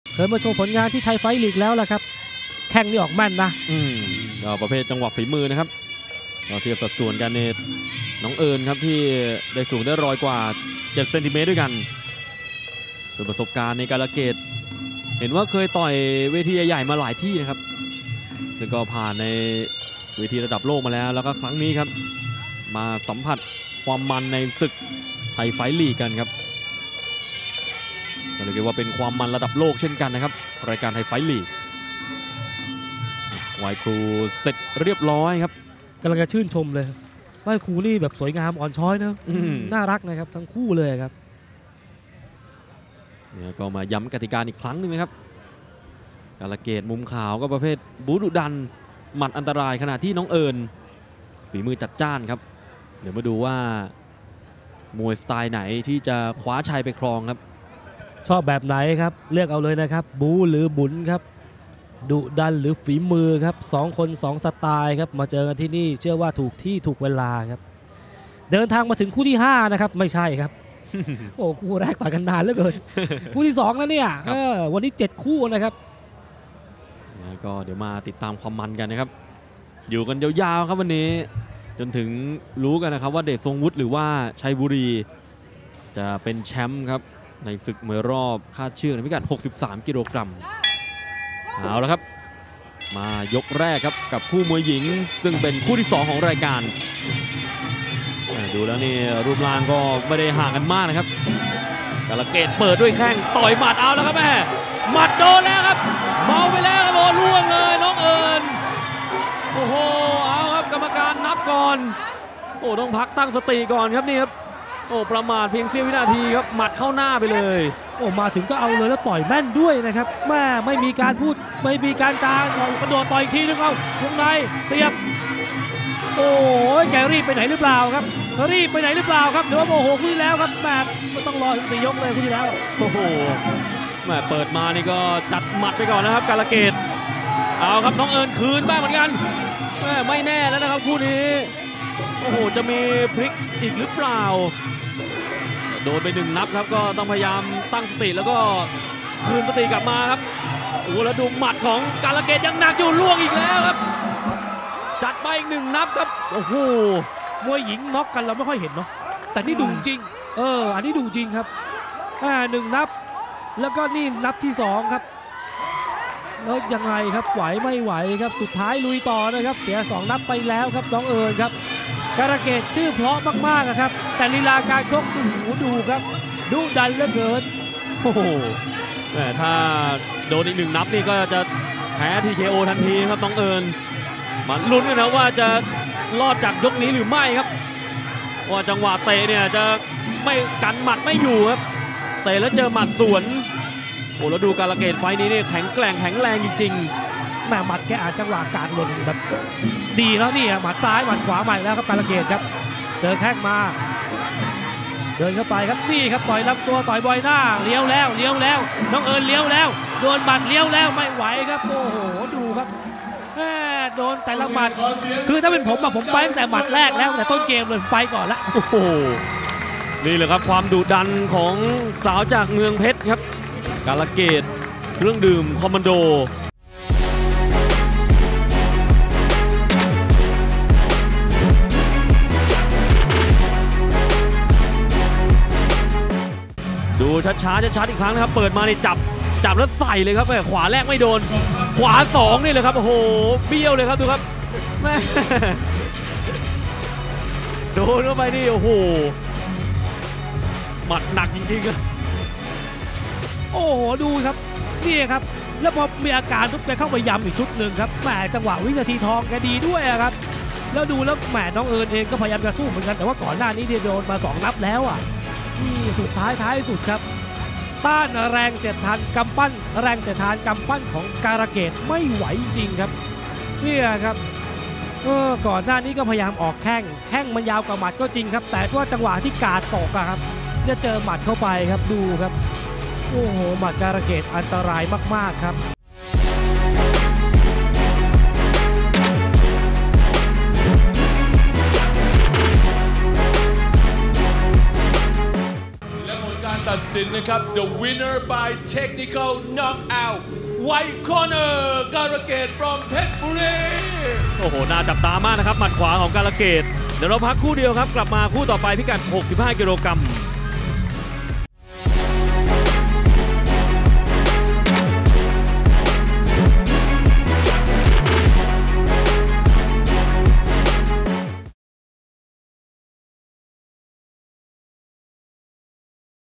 น้องเอิร์น ศ.กัตติกา VS การะเกด เครื่องดื่มคอมมานโด | THAI FIGHT LEAGUE #22 | ไทยไฟท์ลีก #22
THAI FIGHT LEAGUE เวทีจะลุกเป็นไฟ ด้วยคู่ชิงชนะเลิศรุ่น 63 กก. ระหว่าง “เดชทรงวุฒิ ศิษย์ ดร.ชาญชัย” ดวลเดือดกับ “ชัยบุรี ลูกสิงห์นำชัย” พร้อมด้วยไฟท์ที่ 2 รอบรองชนะเลิศในรุ่น 61 กก. “ขุนศึกเล็ก ศิษย์ผู้ใหญ่เทพ” ขอย้ำแค้น “ซุปเปอร์บอล ฉลามศึกมวยไทยยิม” และ “ฟ้าสยาม เดอะเลเจ้นด์อารีน่า” ขอล้างตา “เขี้ยวเพชร เกิยรติไพรสณฑ์” ตามด้วยคู่มวย SUPER FIGHT ทั้งชายและหญิง

วันอาทิตย์ที่ 12 พฤศจิกายน 2566 (ถ่ายทอดสดการแข่งขัน)
🔵 Facebook Live
🔴 LIVE EN YouTube Thai Fight International